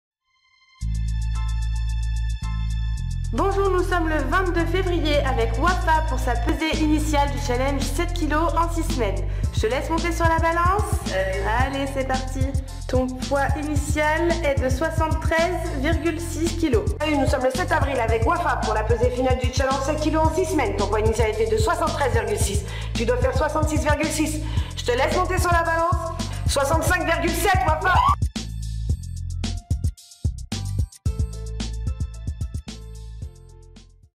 Transformation physique rapide Lyon
Ouafa a perdu 7,900 kgs en 6 semaines à la Training Academy